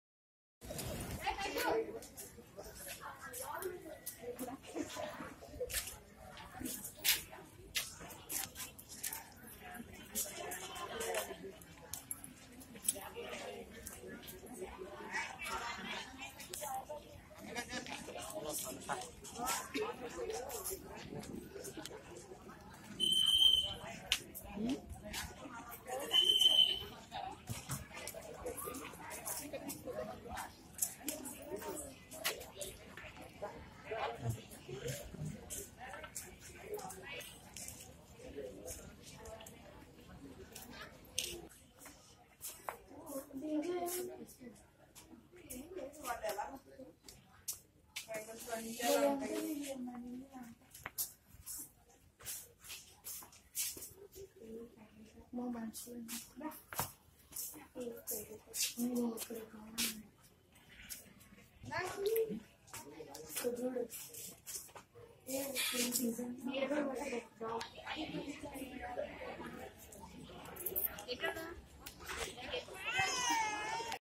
Village Museum/Rural Museum in Shilparamam
The New Way to find our  Shilparamam
Just see from the iconic structure of HITEC city located about 14 kms from Hyderabad at Madhapur. You'll find the impressive gateway with huge terra-cotta horses that leads to a village with festive looking precincts. This is Shilparamam, the arts, crafts and cultural village of Hyderabad.